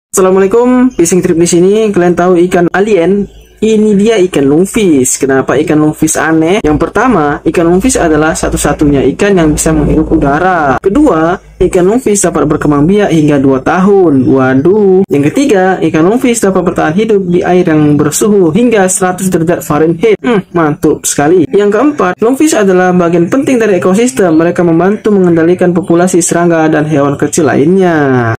Fakta Aneh Ikan Lungfish
Apa saja fakta menarik tentang lungfish?

Berikut adalah beberapa fakta menarik tentang lungfish:

Lungfish adalah satu-satunya ikan yang bisa menghirup udara.
Lungfish dapat berkembang biak hingga dua tahun.
Lungfish dapat bertahan hidup di air yang bersuhu hingga 100 derajat Fahrenheit.
Lungfish adalah bagian penting dari ekosistem. Mereka membantu mengendalikan populasi serangga dan hewan kecil lainnya.

link pembelian